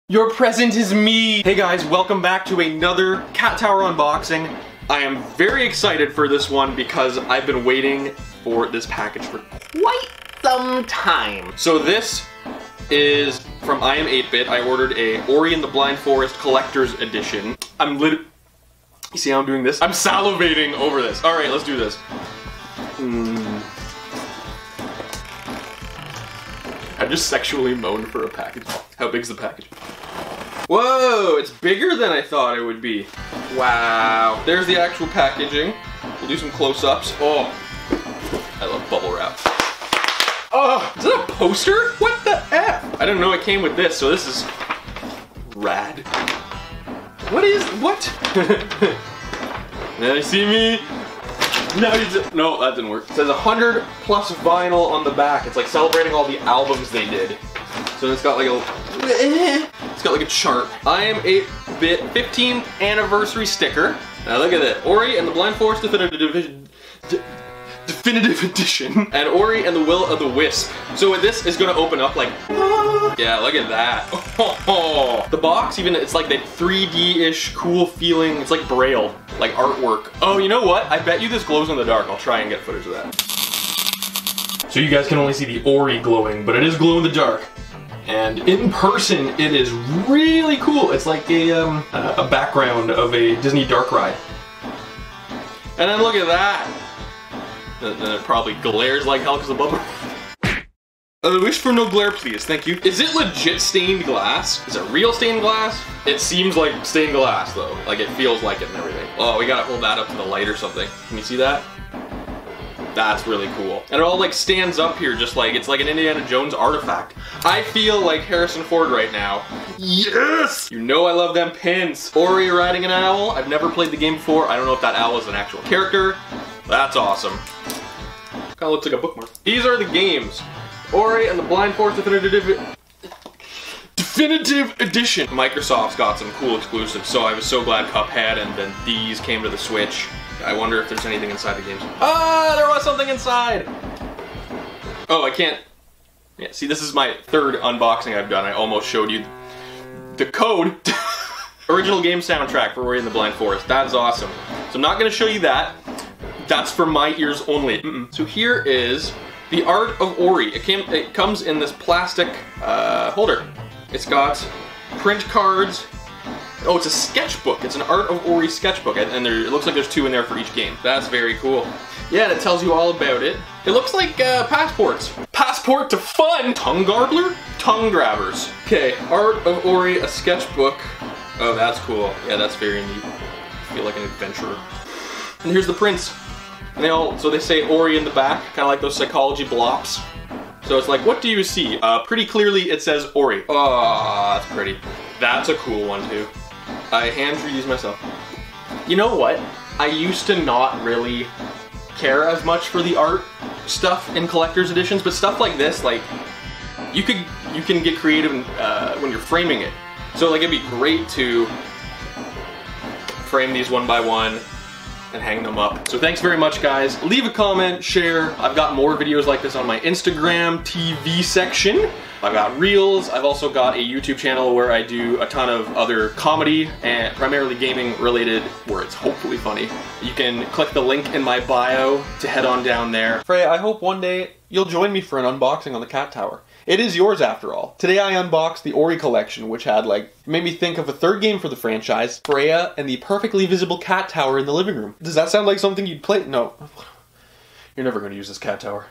Cat Tower Unboxing - Ori & The Blind Forest Special Edition!
Freya and I unbox this beautiful Ori series special edition from Iam8bit!  It's even got STAINED GLASS!!  Now that's just class.